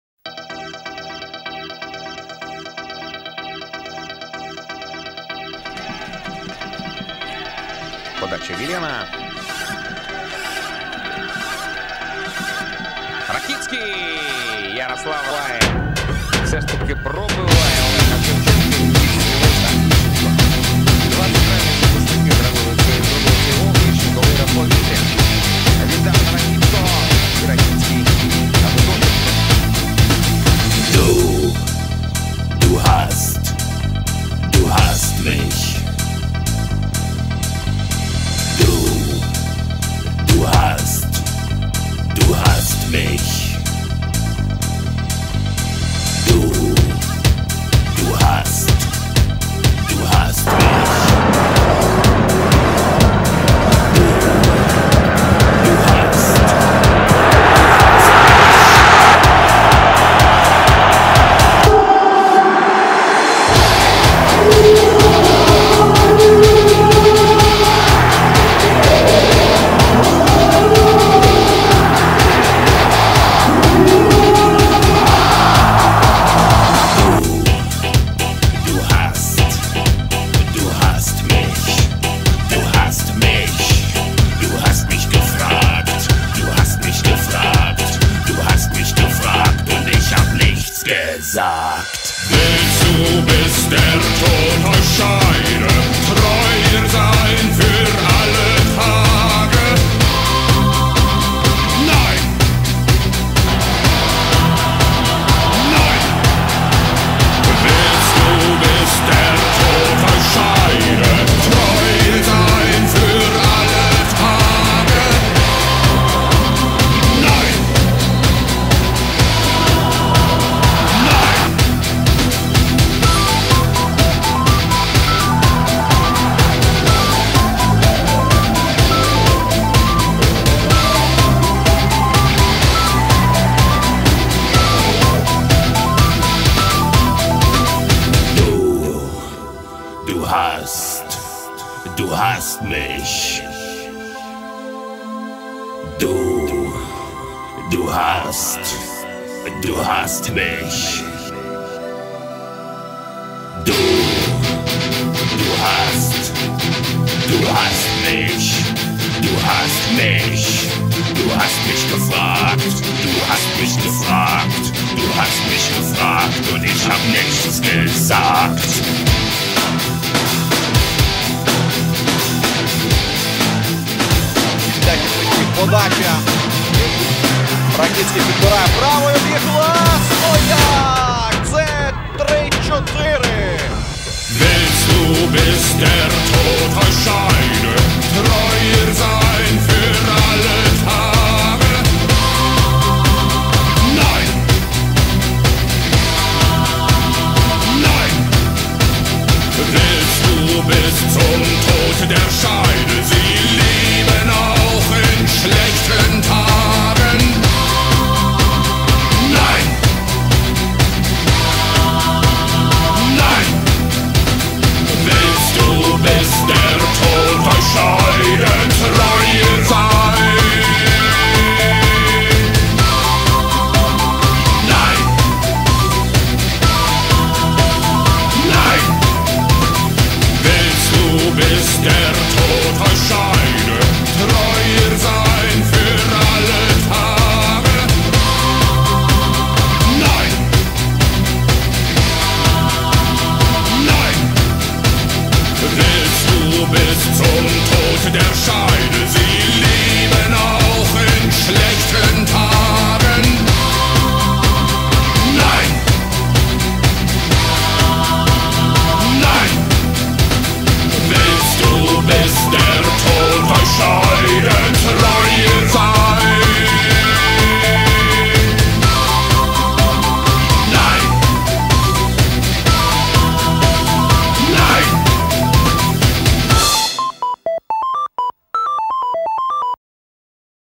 13 голов Ярослава Ракицкого за Шахтер
Смотрите каждый из 13 голов Ярослава Ракицкого за «Шахтер». Какой понравился больше всего?